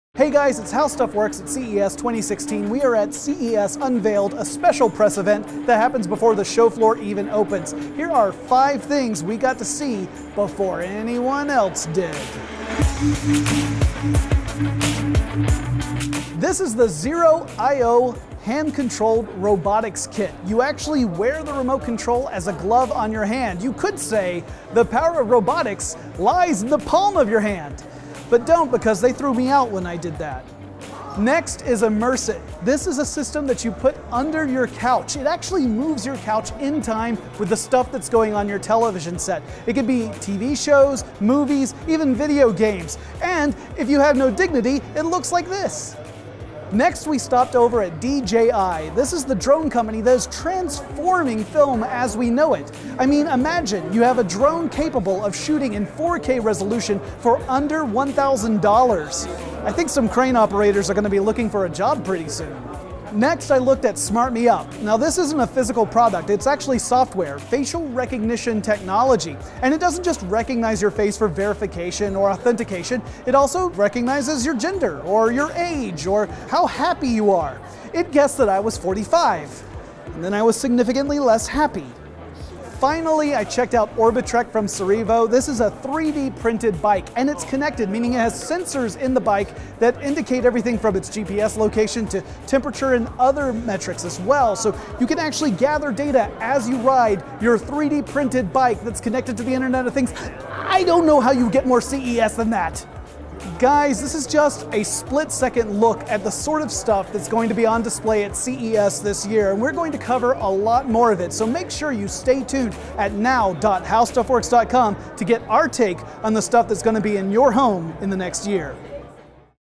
5 Technologies that Caught our Eye at CES 2016 | HowStuffWorks NOW
Before the showroom floor opens at CES, there’s a special event called CES Unveiled, and it gives companies a chance to get their products out in front of the media before the madness of the show really ramps up. HowStuffWorks was there for CES Unveiled 2016 and here’s a wrap up of what we saw.

5 Technologies that Caught our Eye at CES 2016 | HowStuffWorks NOW